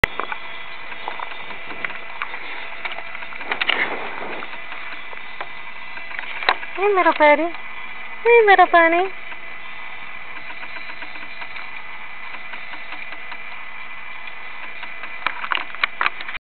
Bunnies in my Garden! Part 2
OK, I said there were 3, my mistake there are 4 baby bunnies in my garden.

So much for my organic garden... now bunny food!